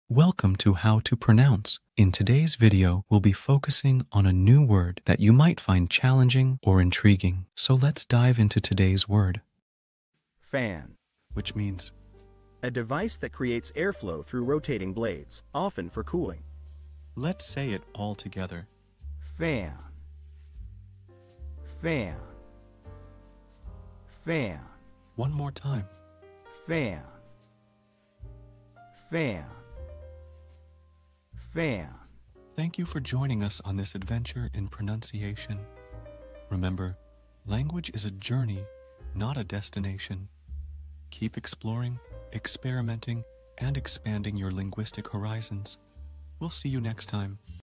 How to pronounce Fan
🌍 Learn a new language now Master the Pronunciation of 'Fan' - which means : A device that creates airflow through rotating blades, often for cooling. 📝 with @HTPronounce 🎓🗣️ - [Your Guide to English]

Learn to pronounce 'Fan' perfectly in English with our easy and engaging tutorial by @HTPronounce! 📖📽️ In this comprehensive video, we provide you with native speaker examples 🗣️👂 and phonetic breakdowns 🎵 to ensure you get the pronunciation just right.

How to say Fan in English